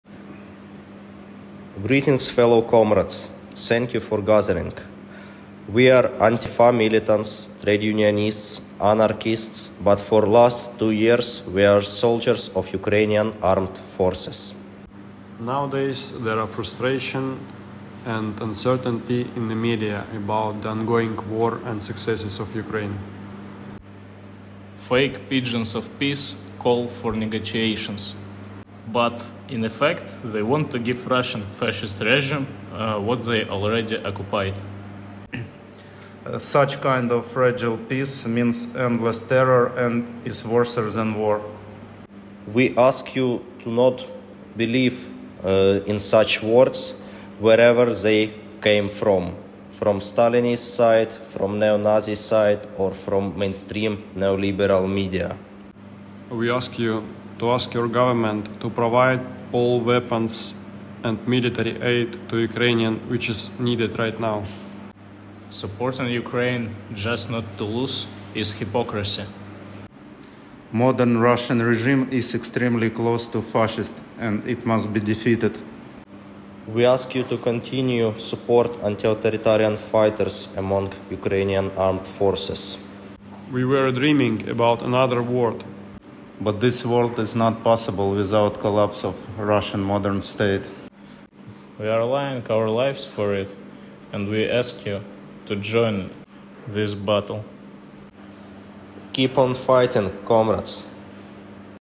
Statement from anarchists in Ukraine fighting against russian invasion on 24 february 2024
Audio address by comrades from Ukraine, now working on various directions, for the anniversary of the full-scale invasion. Recorded originally for anniversary demonstrations across Europe.